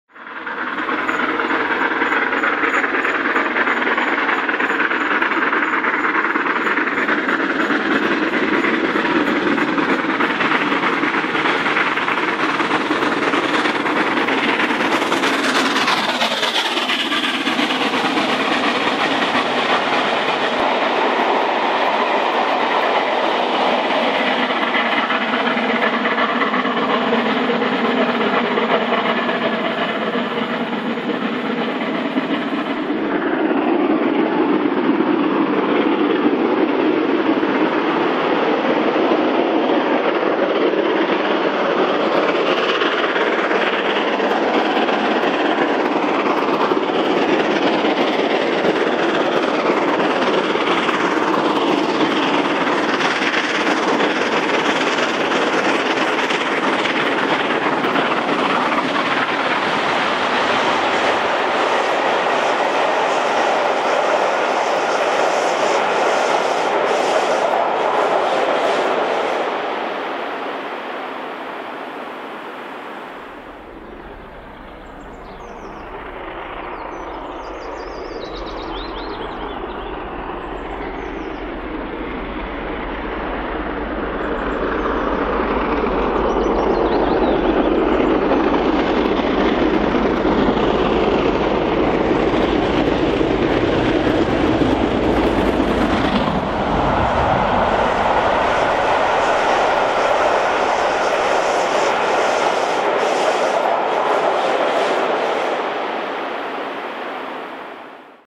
LMS 46115 Scots Guardsman takes on the Devon Banks
The 20th August 2023 saw LMS 7P No.46115 'Scots Guardsman' take on the Devon Banks, hauling 'The Royal Duchy' to Par.

46115 is seen passing Beambridge, Largin Viaduct and Cockwood Harbour, clips taken from our 4-disc review, 'Steaming Around Britain 2023'.

Available in both DVD and Blu-Ray formats, save 10% when ordering online using code: FB2023 [expires 29th February 2024]